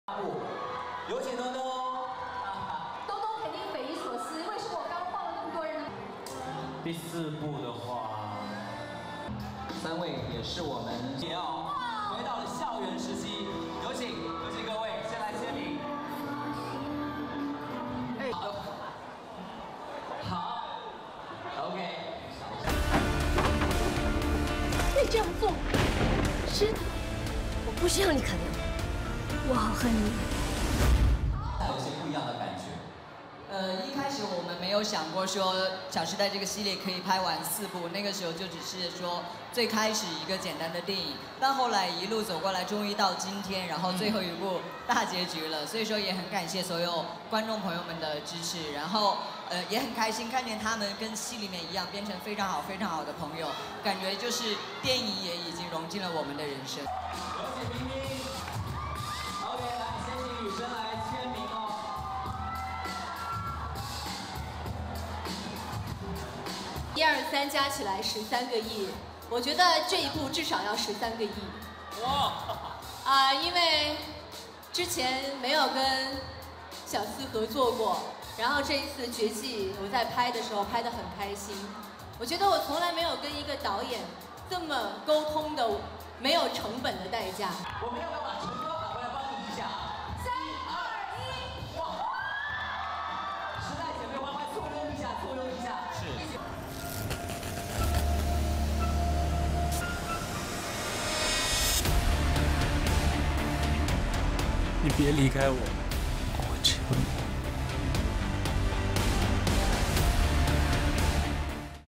Fan Bingbing Walks Red Carpet for 'Tiny Times 4'
Actress Fan Bingbing joins director Guo Jingming and actors Chen Xuedong, Yang Mi, Guo Caijie (Amber Kuo), Xie Yilin and Bea Hayden(Guo Biting) on the red carpet at the premiere of their movie 'Tiny Times 4: Soul's End.' (July 8)